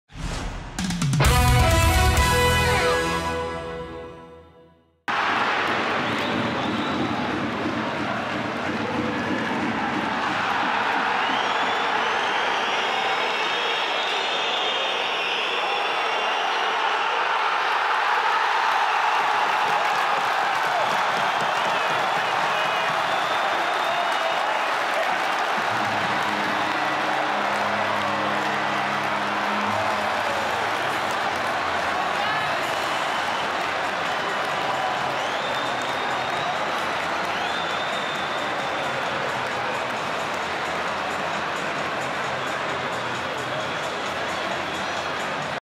L'autogoal gag de FR Gottéron
Niklas Hagman se souviendra longtemps de son premier but avec FR Gottéron